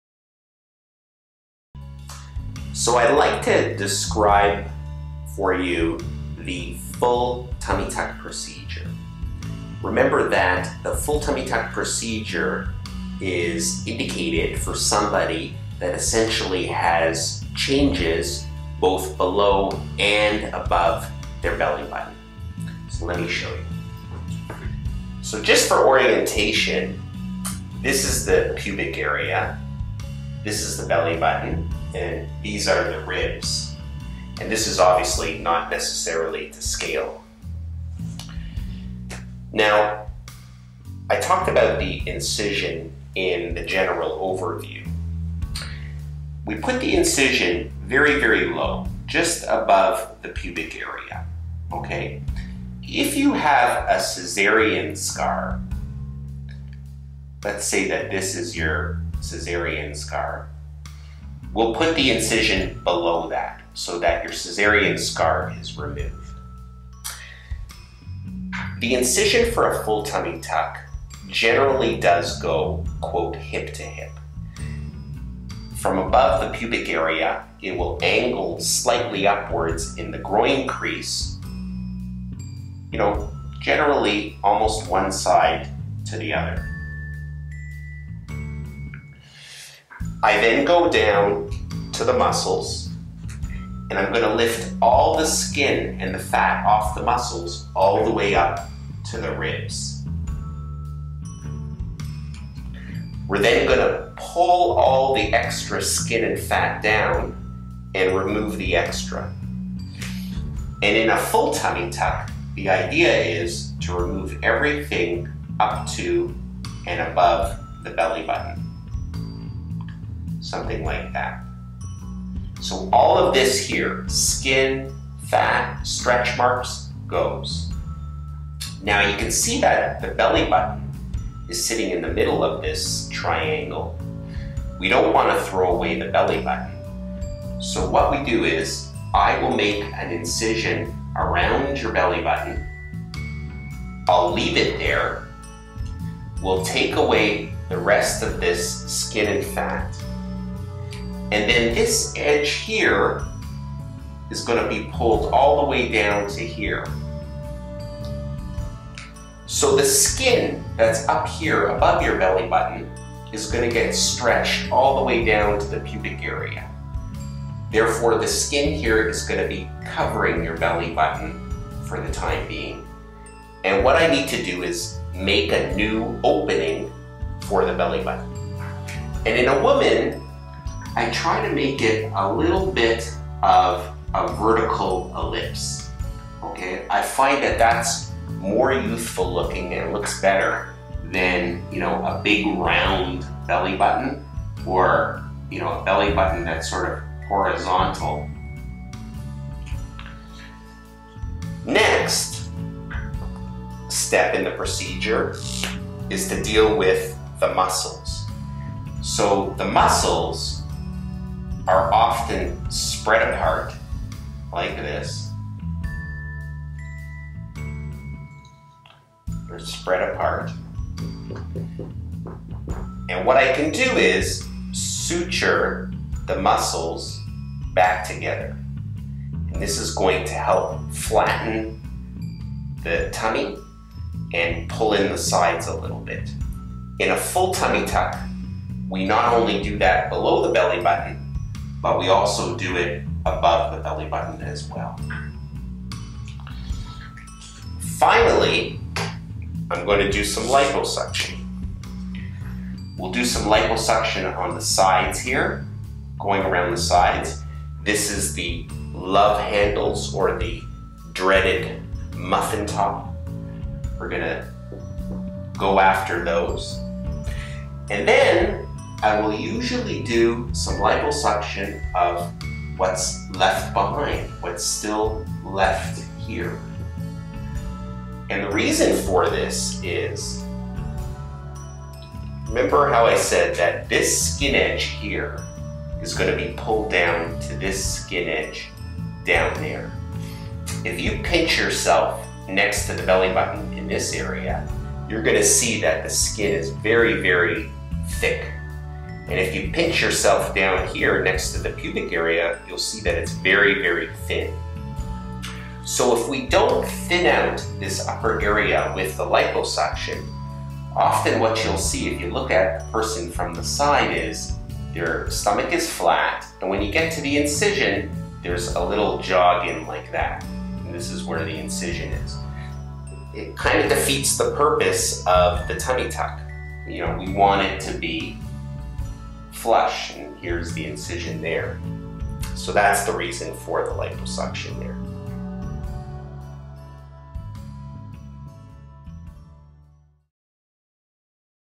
Full Tummy Tuck Explained (Abdominoplasty) - Edelstein Cosmetic - Toronto
Dr. Jerome Edelstein illustrates a full tummy tuck and explains the procedure, when it should be used and how to reconstruct the bellybutton.

Edelstein Cosmetic
362 Fairlawn Avenue,
Toronto, ON, M5M 1T6